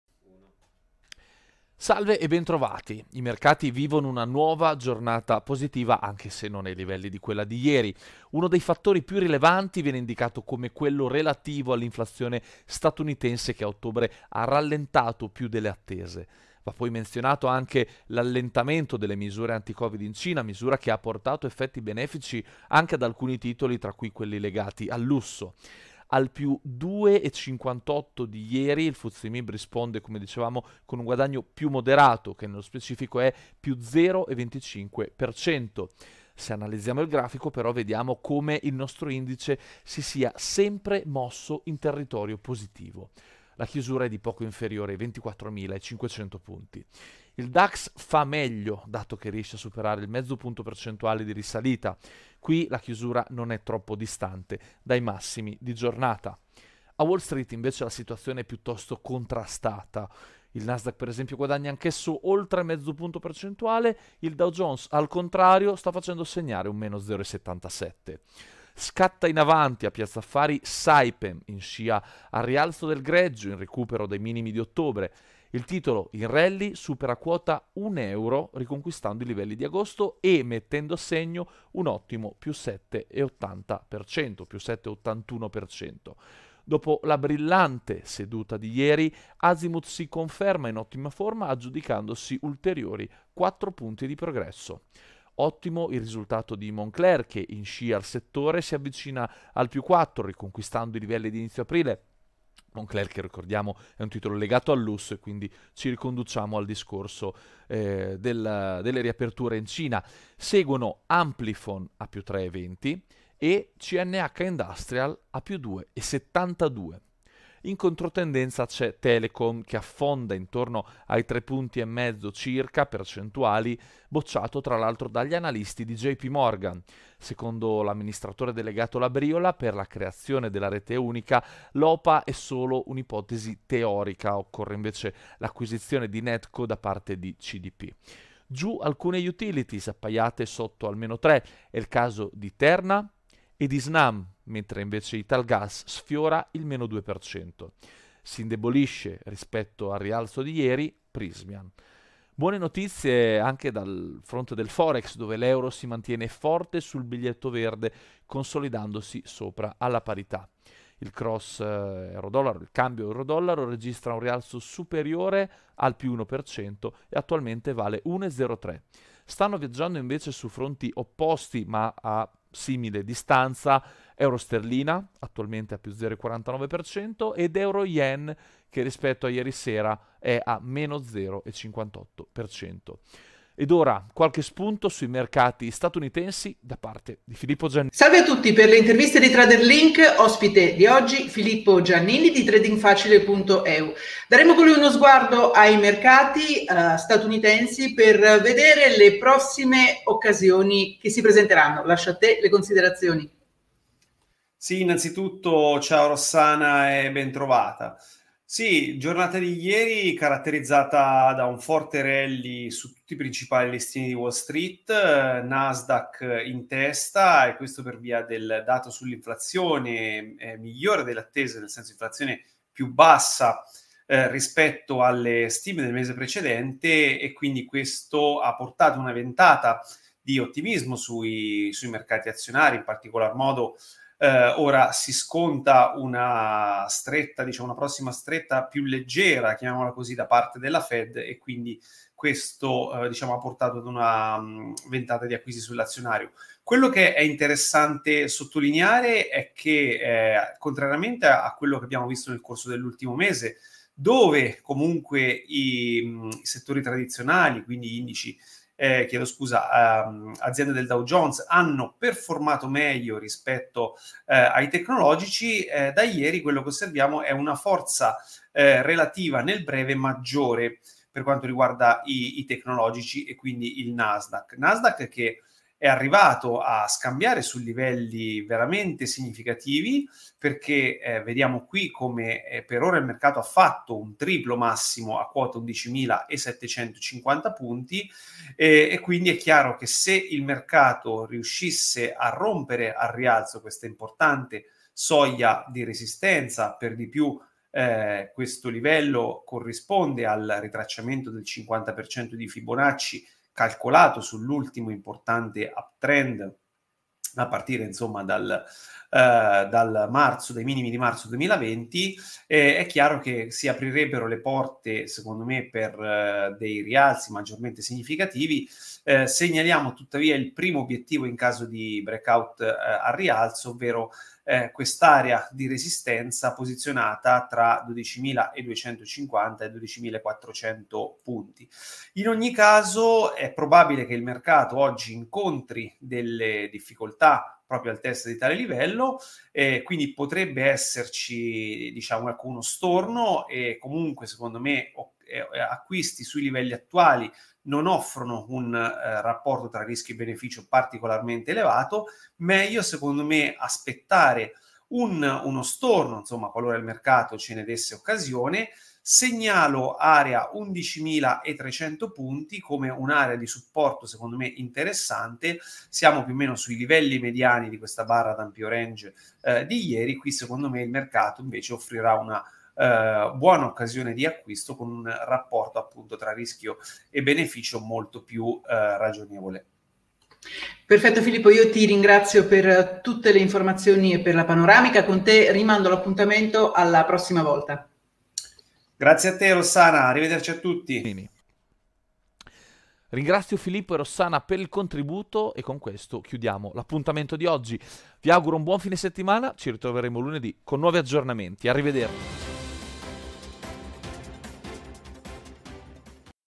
OCCHIO ALLA BORSA - Le news finanziarie dell'11 novembre
Le news finanziarie con il contributo di Filippo Giannini: Le prossime occasioni sui

Dopo il boom di ieri, i mercati europei continuano a salire, anche se in misura minore. Saipem il migliore di tutti, TelecomItalia il peggiore.